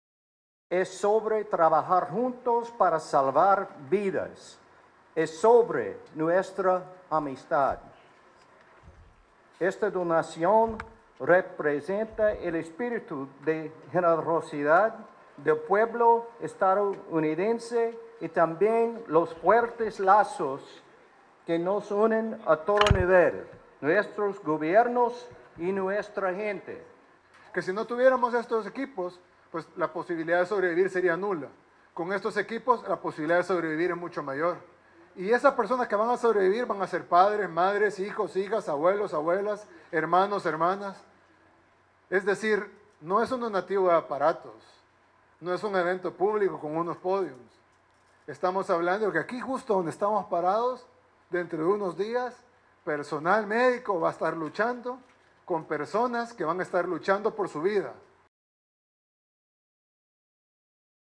Embajador de EEUU entrega donación de respiradores mecánicos
El presidente Nayib Bukele y el embajador de Estados Unidos, Ronald Johnson, entregaron al sistema de salud de El Salvador 250 respiradores mecánicos para la atención de pacientes con covid 19.